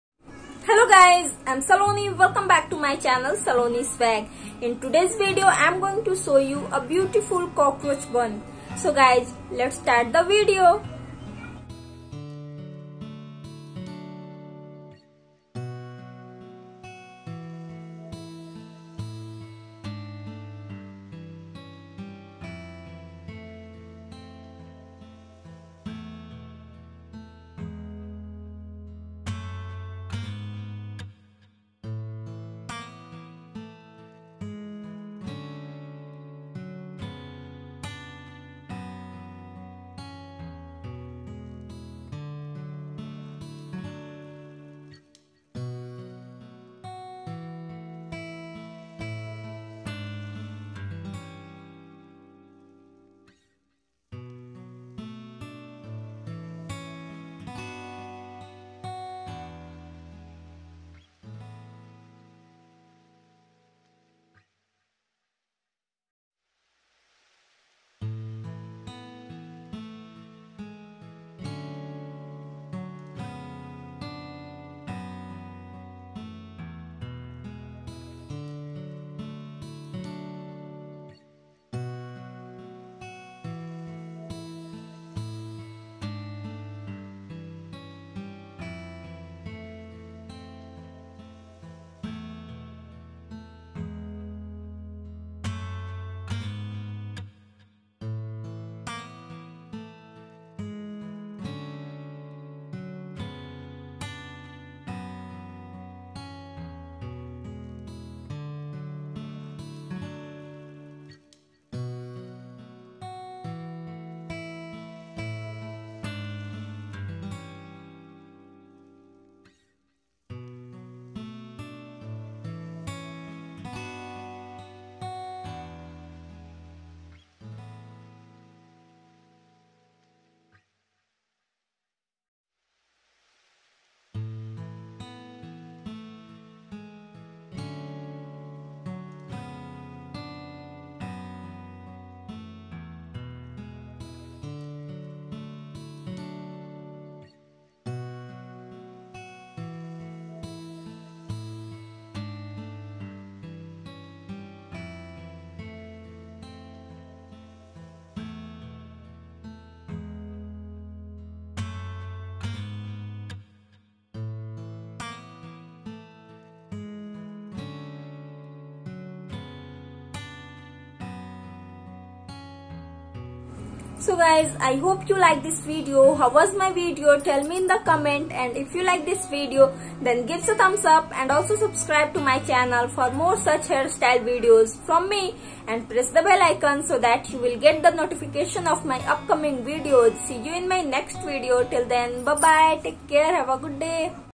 Easy party cocroch Bun | new latest unique Hairstyle Tutorial | Beautiful Bun| Saloni Swag
Hello guys!!

In this video i show you a Beautiful Easy cocroch Bun for party/ shopping/college / everywhere /everyday...i hope you enjoy this video...

iss video me mai apko btaungi khule balo ka hairstyle jo ap party /shopping /college ya kahi v kabhi v kar skte ho... iss video ko dekhne k baad ap bhut hi aasani se apna khud ka hairstyle khud kr skte ho. ye vdo beginner k kafi jada helpful hai.. i hope apsab iss video ko enjoy kroge..

how to make simple hairstyle.
how to make cocroch bun hairstyle.
how to make easy and simple hairstyle.
how to make open hair hairstyle with 2 hair pin.

Love ~
      Saloni Swag